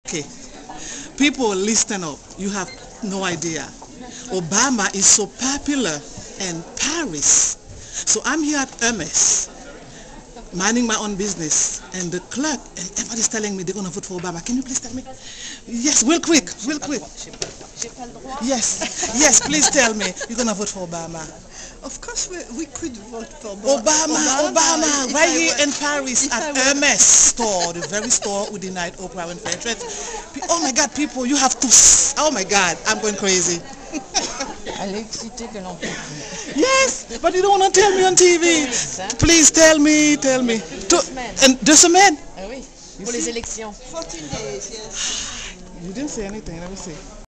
This is the Dept Store ( Hermes) that Denied Oprah Winfrey Entrance
I went to Hermes Paris  after they've denied Oprah Winfrey entrance and I wanted them to tell me that they would've  voted for Obama if they were in the US.